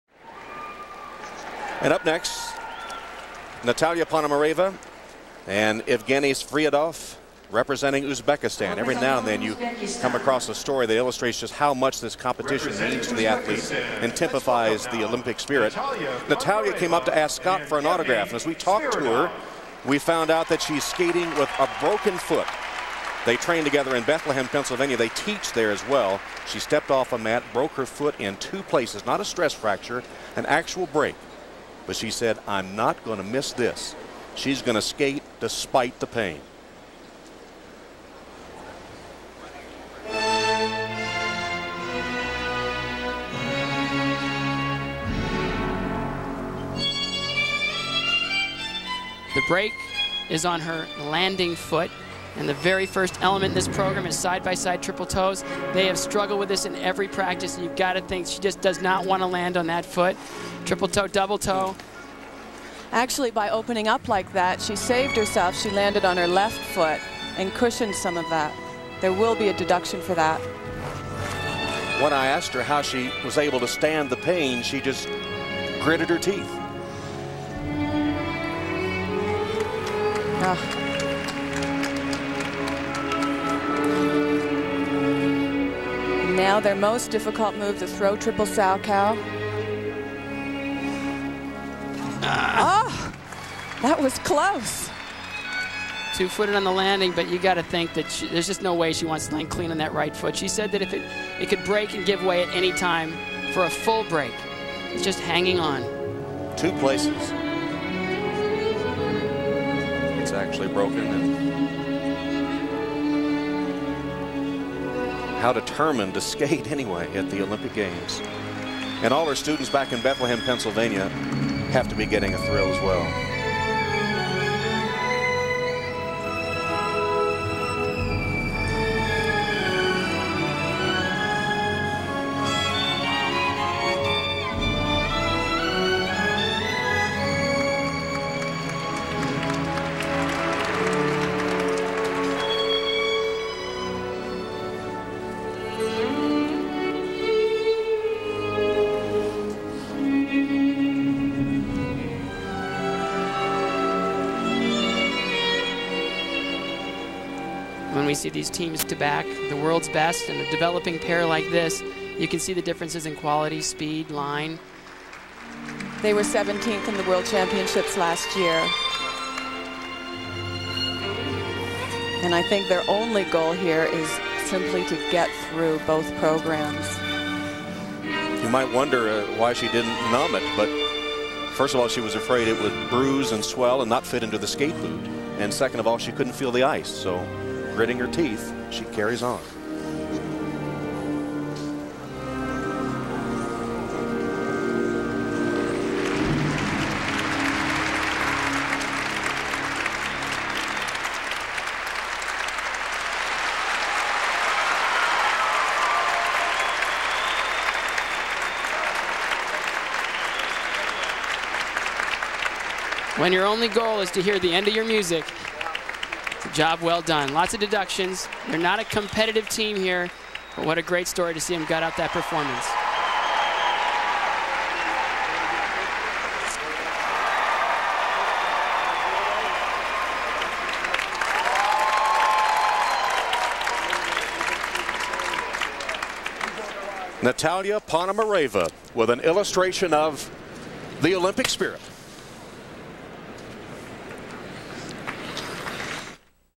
Ponomareva and Sviridov 2002 Olympics Short Program
Natalia Ponomareva and Evgeni Sviridov at the 2002 Olympics in Salt Lake City, Utah.  Saturday, February 9, 2002.  Pairs short program.